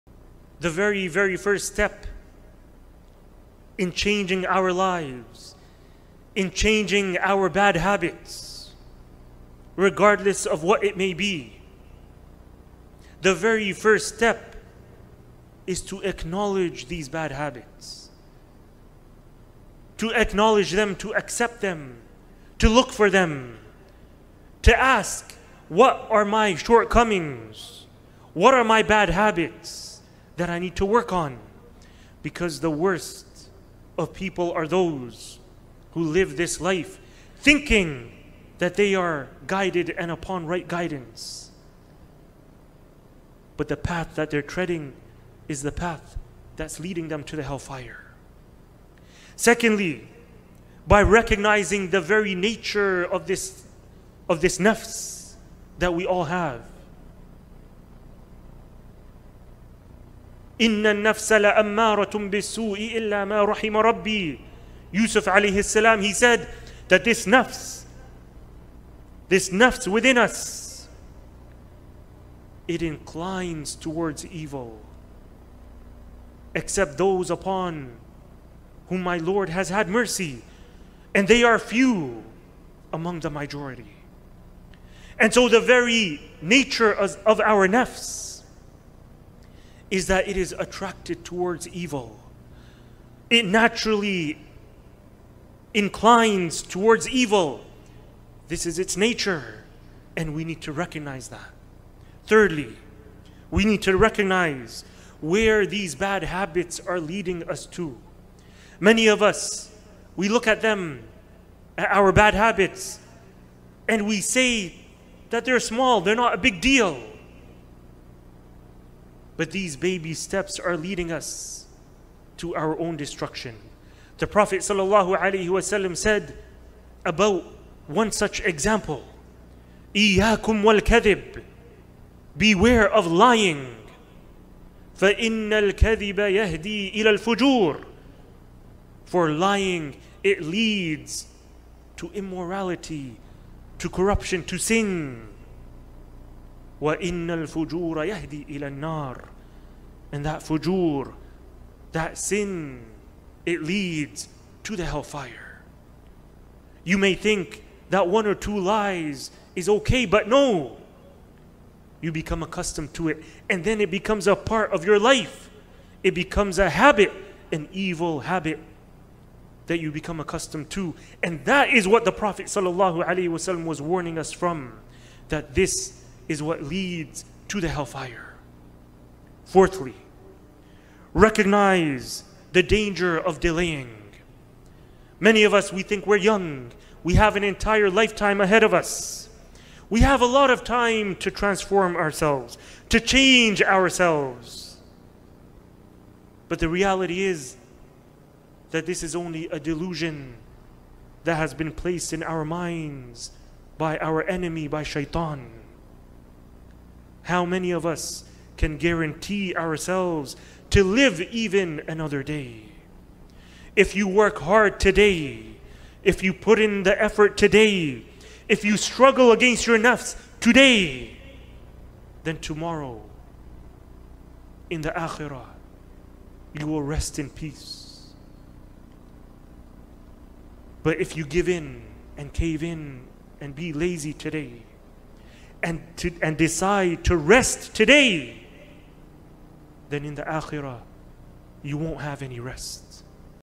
4 Ways To Change Your Bad Habits | Sh. Kamil Ahmad
"Indeed, Allah will not change the condition of a people until they change what is in themselves." {13:11}